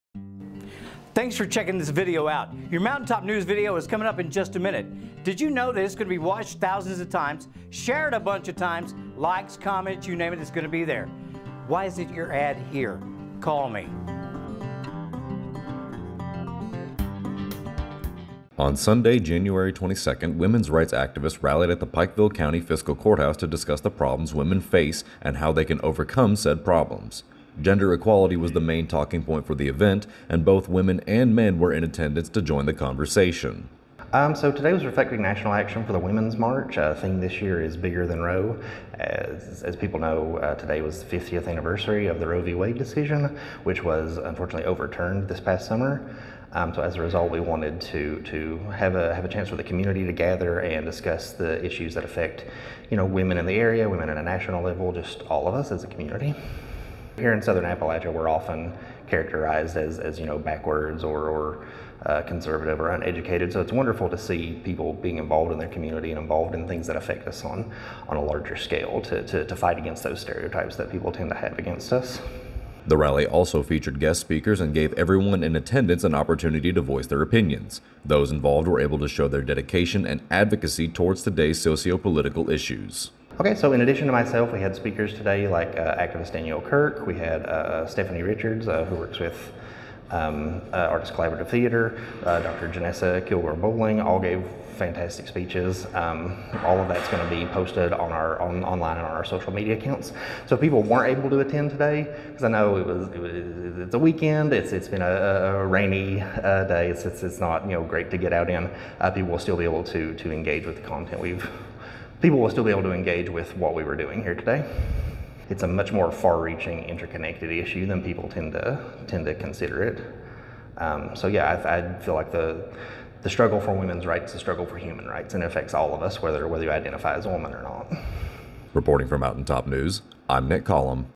Women's Rally Held in Pikeville
A women's rally was held in Pikeville on Sunday, January 22.